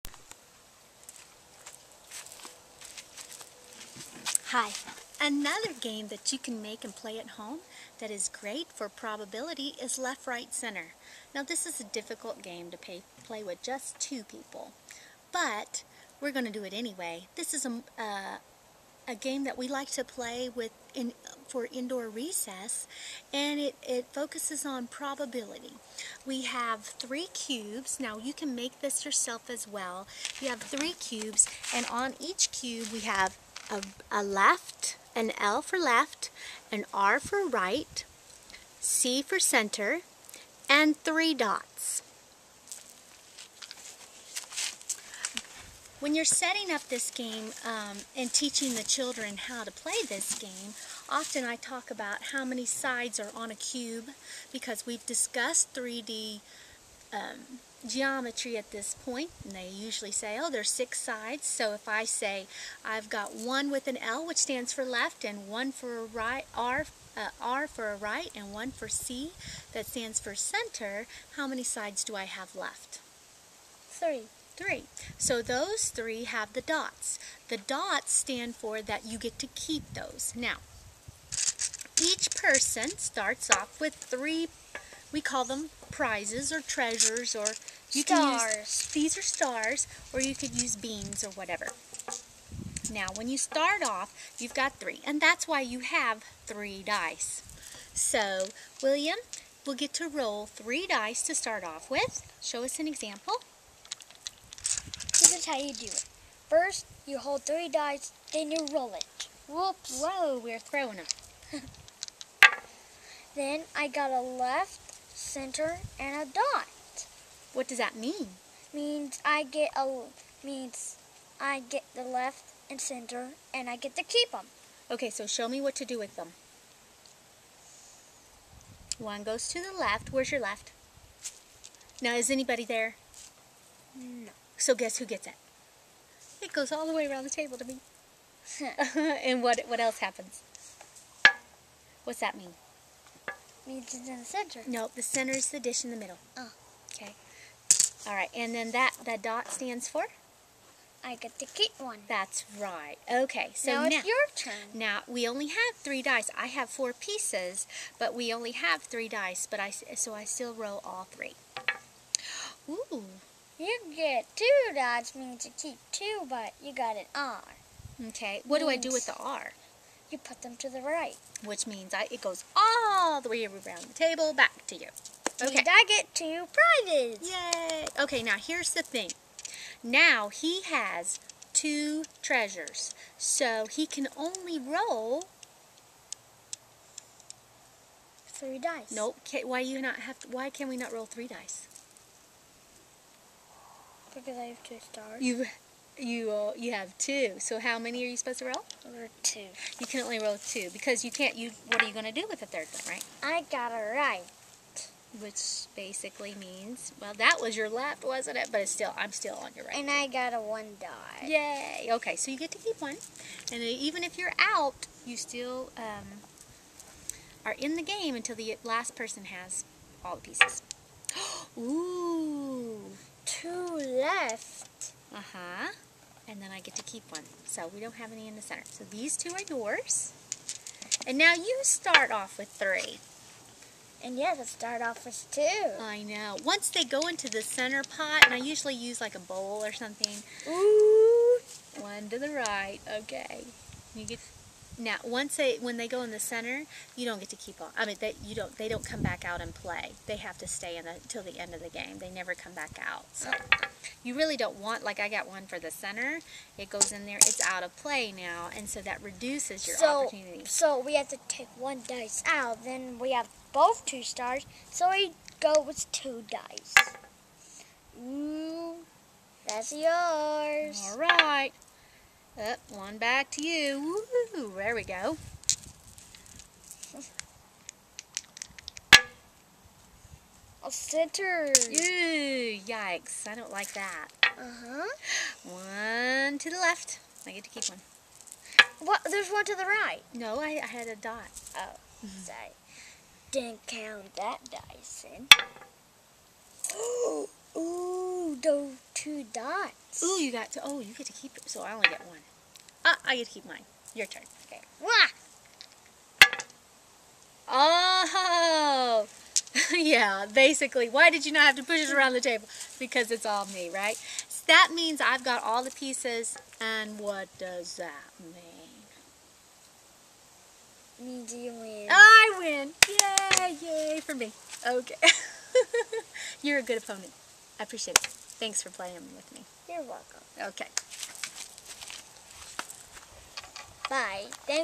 Probability Game for All Ages - Left, Right, Center K12
The Left, Right, Center is a game that can be easily made for any classroom or home. Good for discussion over probability with older students and direction for younger students. Fun way to spend indoor recess too!